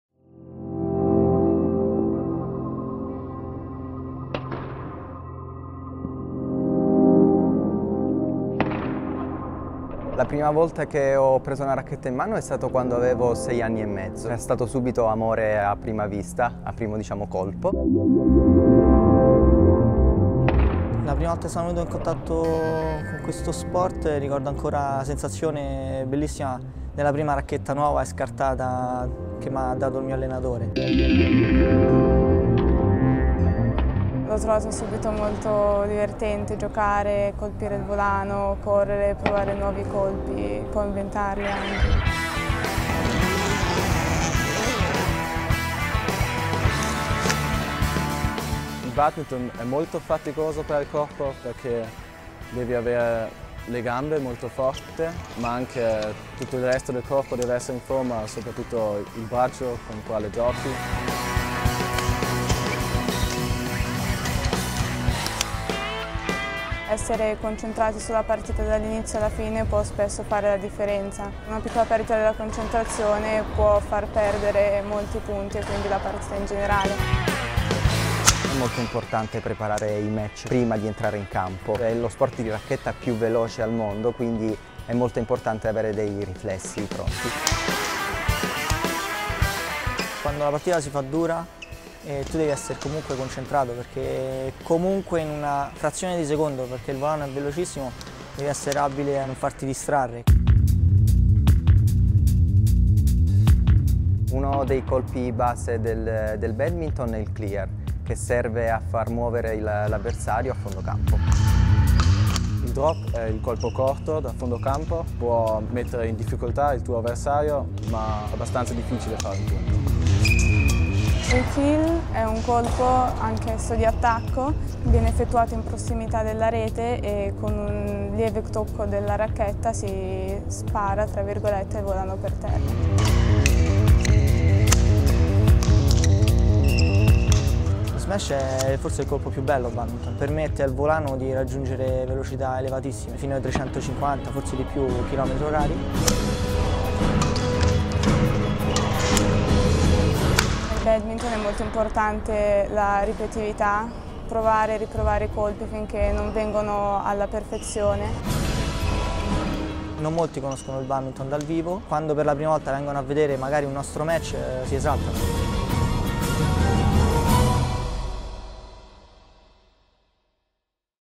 Il Badminton
Tra i più veloci sport di racchetta, il badminton è uno sport in cui si rendono necessarie prestanza fisica, agilità e prontezza di riflessi. L'effetto è spettacolare con scambi rapidi, cambi di fronte e movimentati recuperi. Possono sfidarsi, oltre a due giocatori (singolare), anche due coppie dello stesso sesso (doppio maschile o doppio femminile), o due coppie formate da giocatori di entrambi i sessi (doppio misto).

Il badminton è uno sport praticato in singolo o in coppia su di un campo rettangolare diviso da una rete di altezza 1,55 m.

Il gioco consiste nel colpire un volano con una racchetta facendogli oltrepassare la rete e mandandolo nella metà campo opposta dove dovrà essere ribattuto al volo dall'avversario.